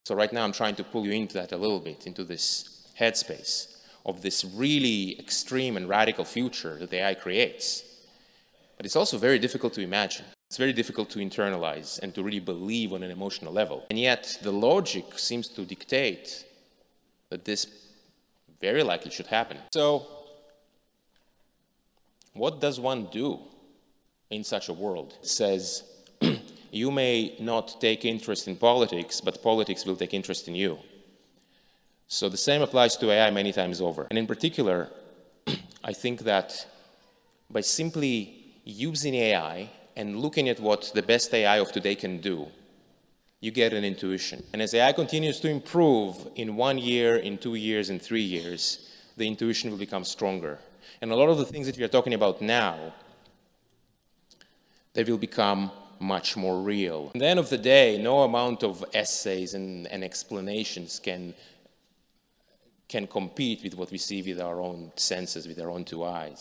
Future AI: Hard to Imagine, But Will Change Your Life!
AI's rapid evolution demands attention. What seems radical today will soon become reality. Using AI provides intuition; witnessing its progress makes the future undeniable. Essays can't compete with direct experience.